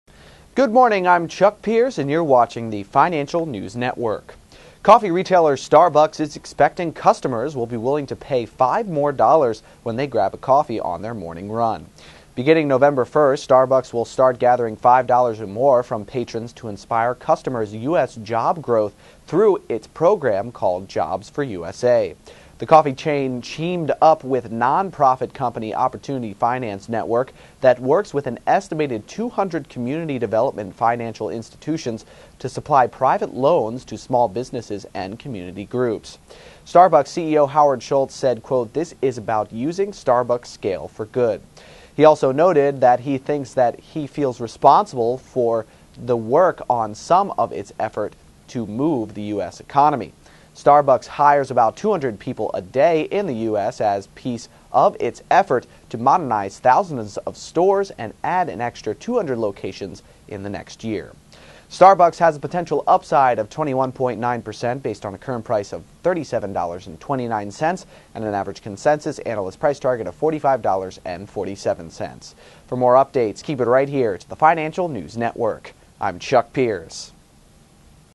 Starbucks Demands Customers to Pay an Extra $5 for Job Growth
Coffee retailer, Starbucks (NASDAQ:SBUX) is expecting customers will be willing to pay $5 more when they grab a coffee on their morning run.

Beginning November 1, Starbucks will start gathering $5 or more from patrons to inspire customers U.S. job growth through its program called "Jobs for USA." The coffee chain teamed up with non-profit company, Opportunity Finance Network that works with an estimated 200-community development financial institutions to supply private loans to small businesses and community groups.

Starbucks CEO Howard Schultz said, "This is about using Starbuck's scale for good." He also noted the he thinks that he feels responsible for to work on something of its efforts to move the U.S. economy. Starbucks hires about 200 people a day in the U.S. as piece of its efforts to modernize thousands of stores and add an extra 200 locations in the next year.

Starbucks (NASDAQ:SBUX) has a potential upside of 21.9% based on a current price of $37.29 and an average consensus analyst price target of $45.47.